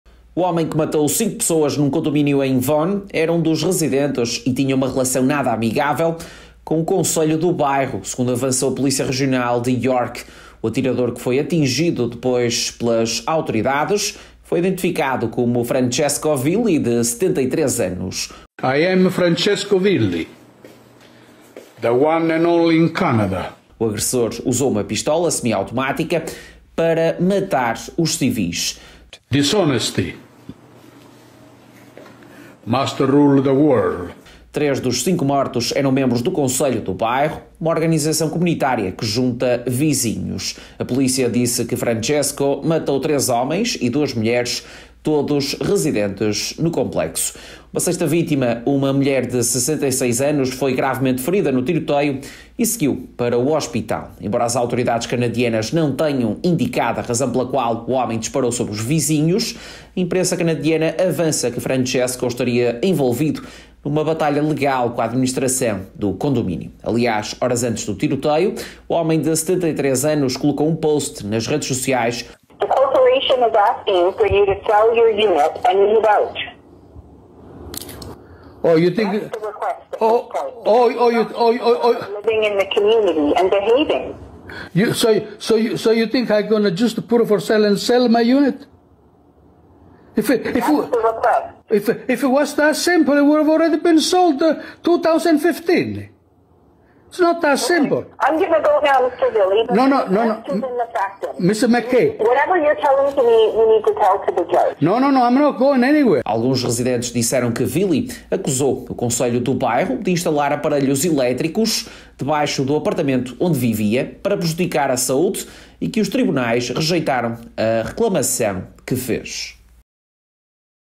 Tiroteio em Vaughan - Francesco Villi, de 73 anos, tem historial conflituoso
O atirador, que morreu num confronto com a polícia em Vaughan, foi identificado como Francesco Villi, de 73 anos. Uma alegada batalha judicial entre vizinhos terá motivado o tiroteio que fez cinco mortos. Mais tarde, Francesco acabaria por ser morto pela polícia.